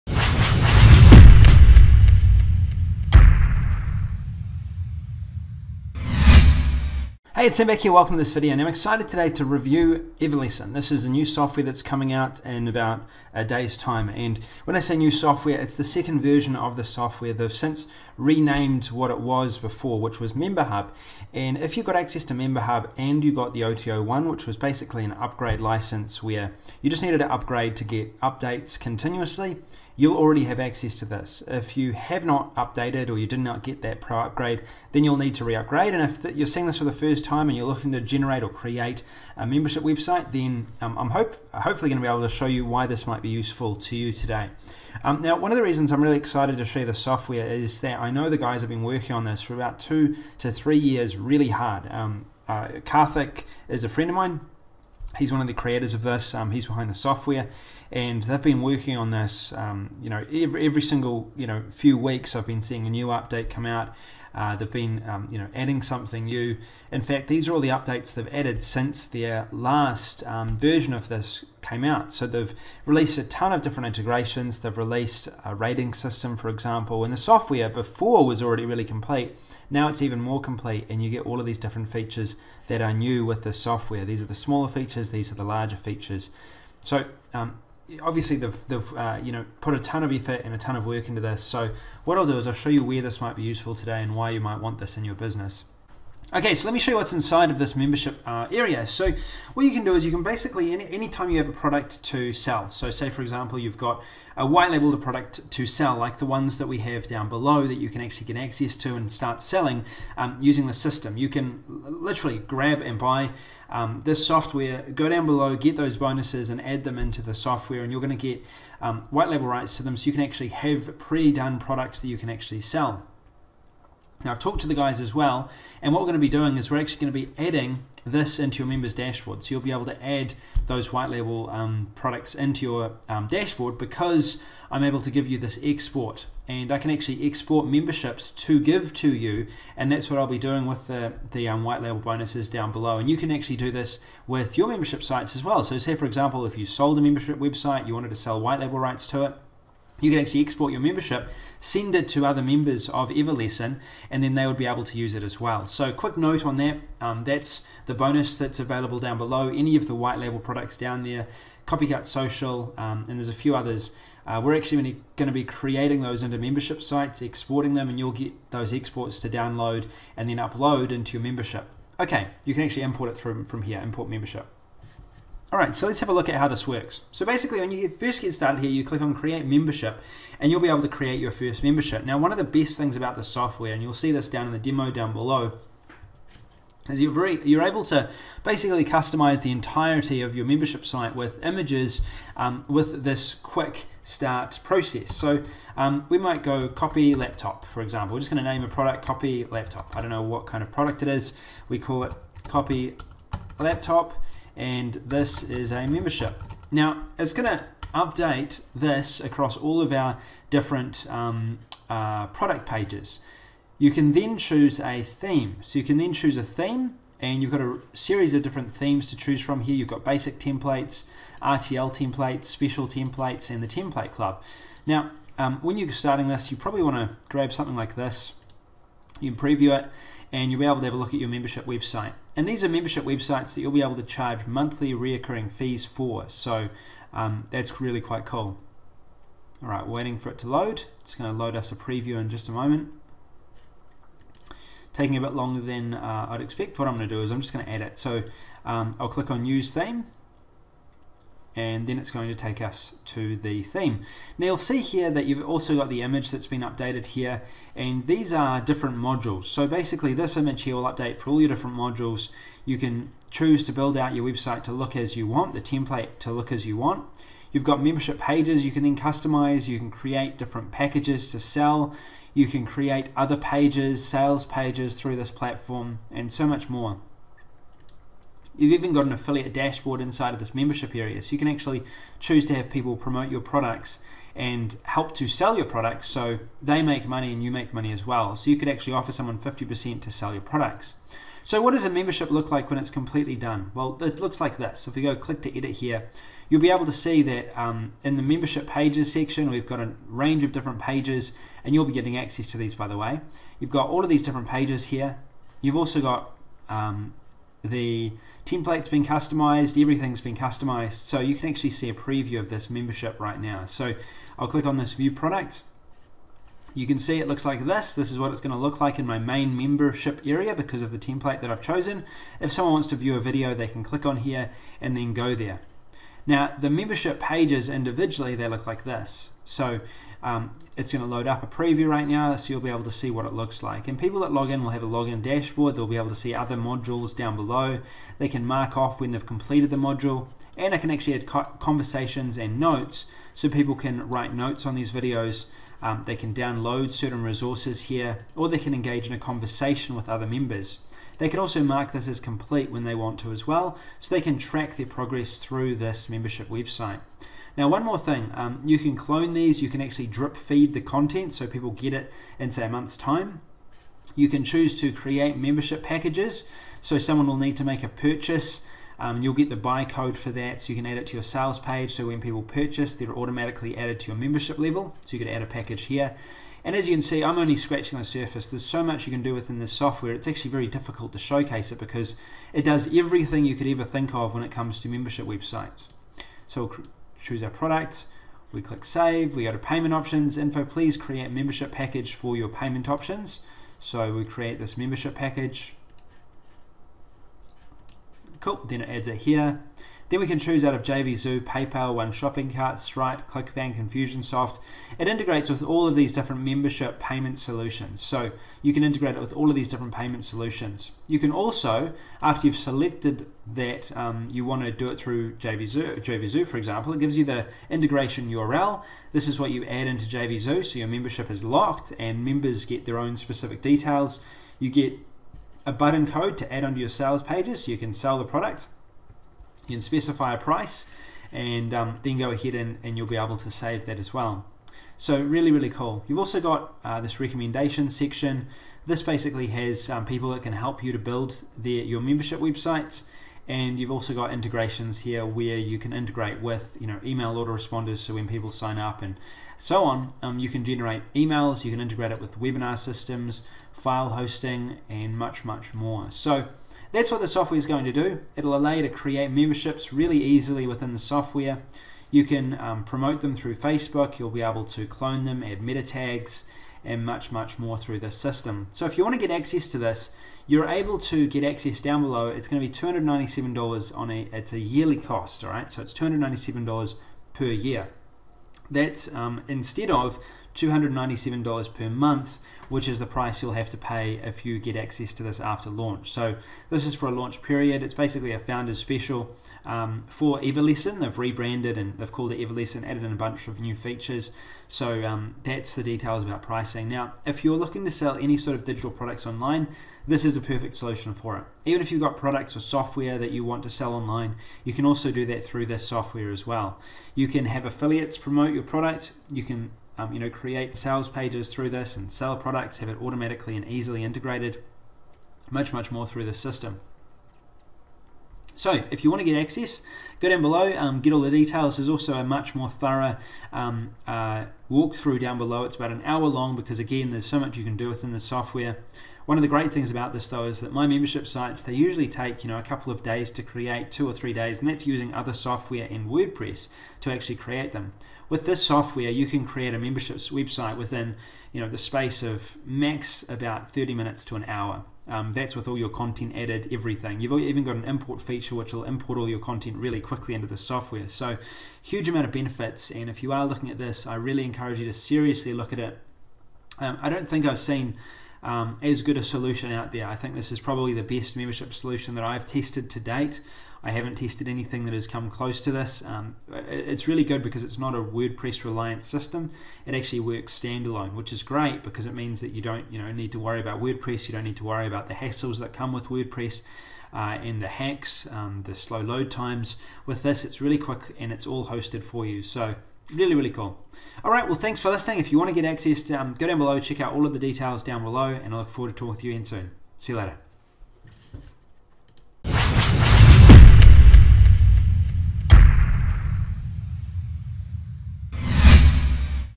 Everlesson Review - Tutorial and Overview - Get your Everlesson Bonuses
Everlesson is a great tool to help you create a membership site very fast and easy. You simply go through many of the themes they have available, then customize the templates to your liking so it looks exactly how you want it to look. Build pages by loading videos, images, text or any other content you want. You can offer your content as packages for your members to purchase. When your info is all ready to go, the site creates its own sales page for you, without you doing anything else.

This is a great new software that will definitely make your life a lot easier.